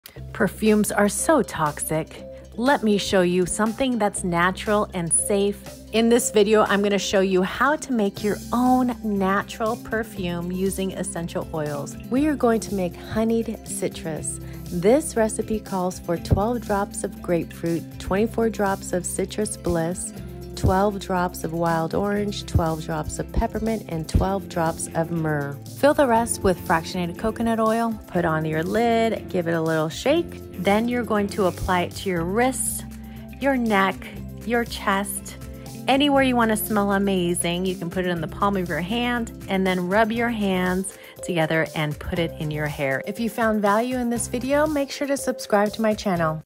Ditch Your Toxic Perfumes | DIY Essential Oil Perfume | Honeyed Citrus
In this video, I will show you how to make Honeyed Citrus Perfume using Essential Oils.  I only use doTERRA Essential Oils.  Quality Matters when it comes to essential oils.  There are a lot of toxic, harmful essential oils in the market.  doTERRA essential oils are certified pure therapeutic grade.

The recipe calls for:
(10 ml roller bottle)
12 drops of Grapefruit Essential Oil
24 drops of Citrus Bliss Essential Oil
12 drops of Wild Orange Essential Oil
12 drops of Peppermint Essential Oil
12 drops of Myrrh Essential Oil
Fill the rest with Fractionated Coconut Oil

to get the highest quality essential oils in your home today!